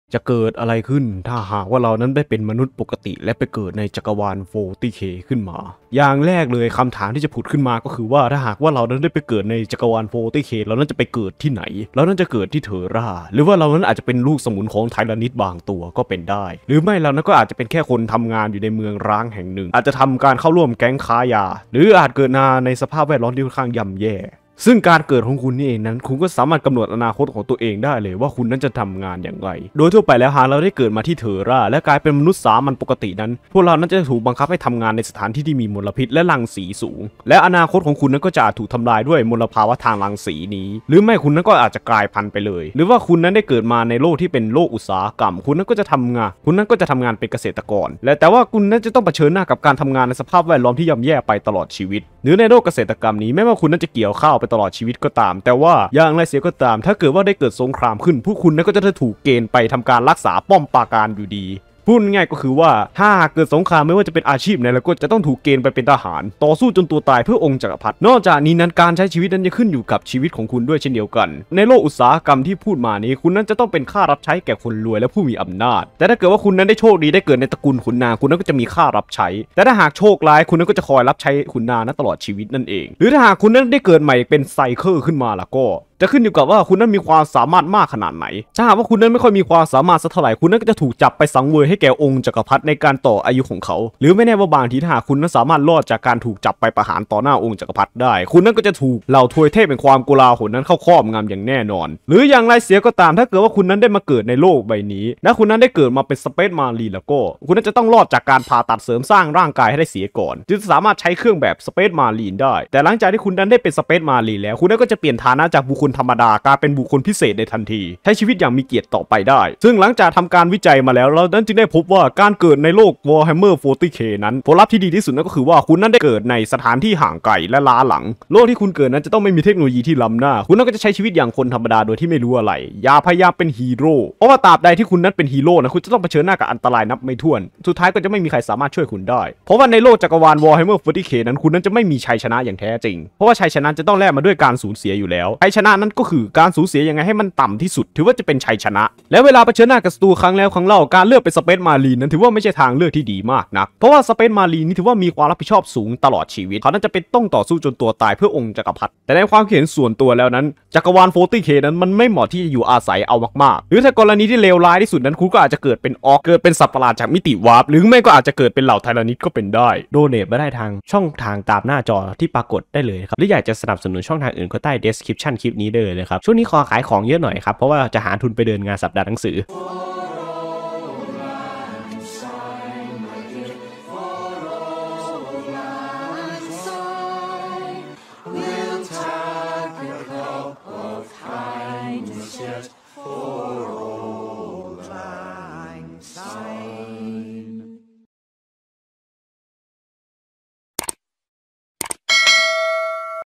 เรื่องเล่าจาก Warhammer 40K : จะเกิดอะไรขึ้นถ้าเราไปเกิดในจักรวาล 40K
คลิป เรื่องเล่าจาก Warhammer 40K : จะเกิดอะไรขึ้นถ้าเราไปเกิดในจักรวาล 40K นี้เป็นคลิปเนื้อเรื่องเสริม ข้อมูลต่างๆของ อาวุธ ตัวละคร เหตุการณ์ Side story  คลิปที่ 29 หากเนื้อหาผิดพลาดประการใดเขียนแก้ได้เลยนะครับ

True Wallet : SpotWorld1
กสิกรไทย : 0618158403